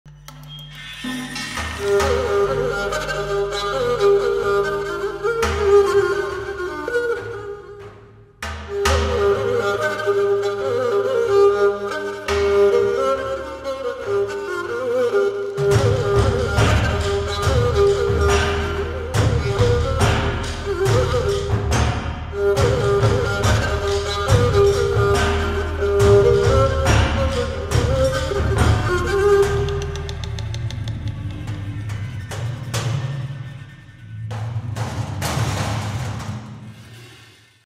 Tai Chi and Tea Ceremony
What an unforgettable ChineseNewYear celebration! 🇨🇳
From mesmerizing performances to a peaceful tai chi session and traditional tea ceremony, we embraced the new year with grace and joy!